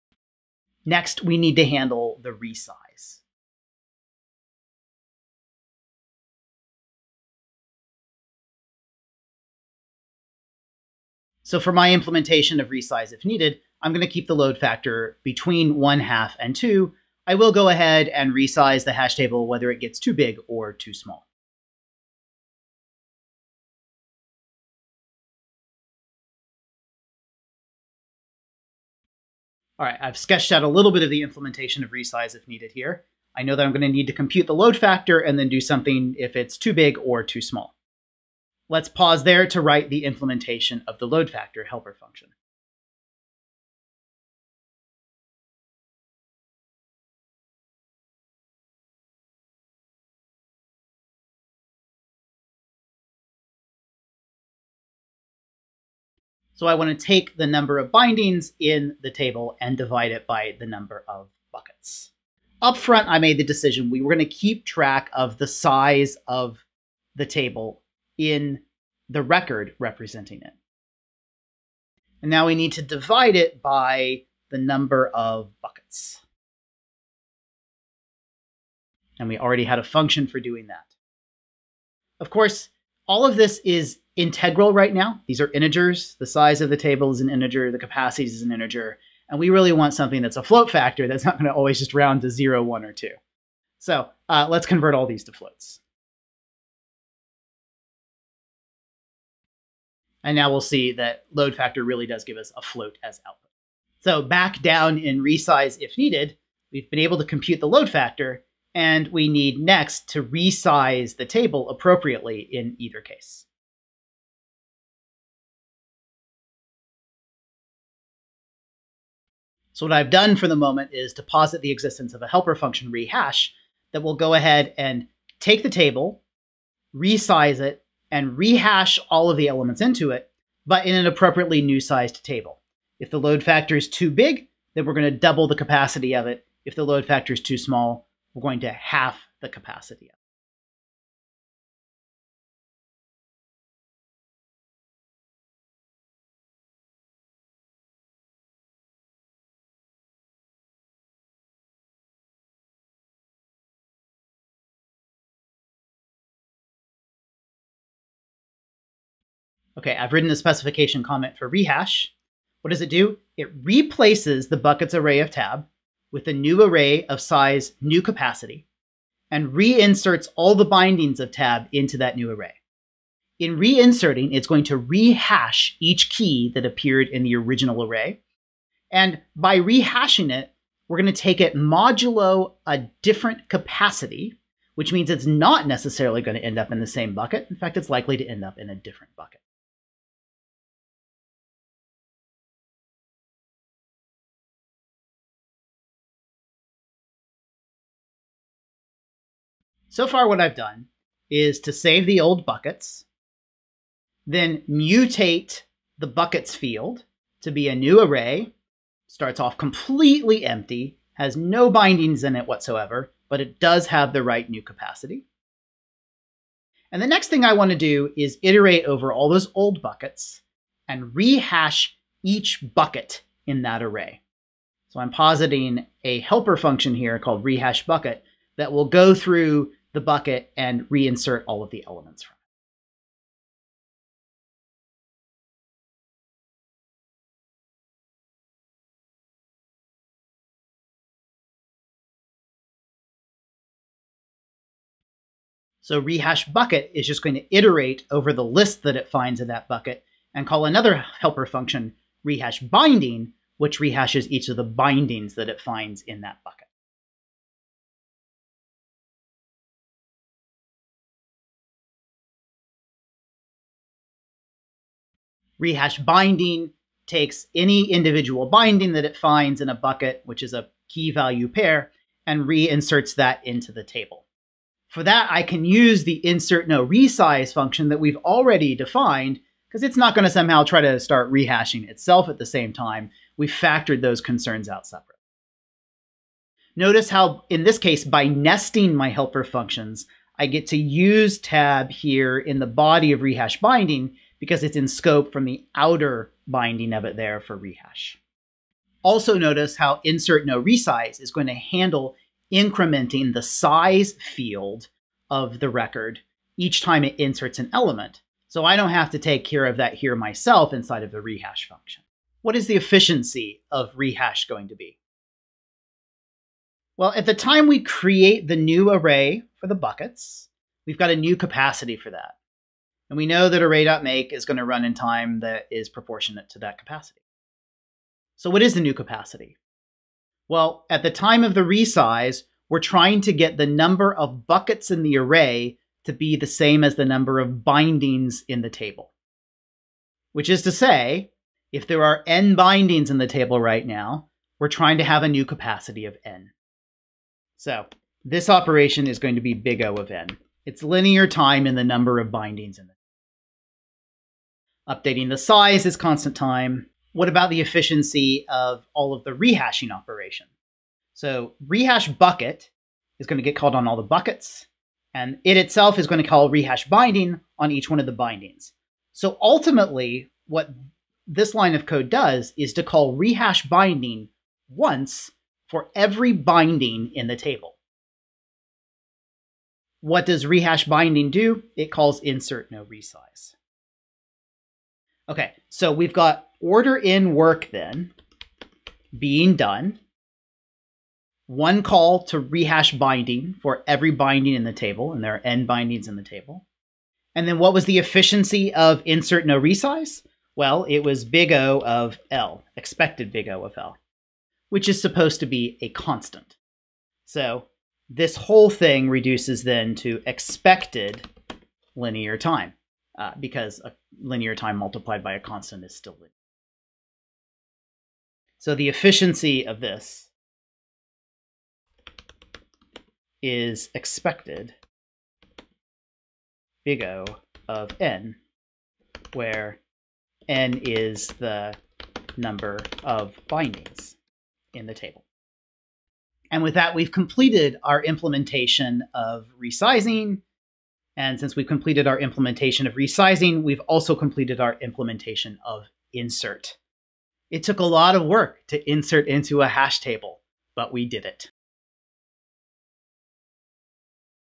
Hash Table Resize Implementation | OCaml Programming | Chapter 8 Video 17
How to implement the resize portion of the insert operation for hash tables with chaining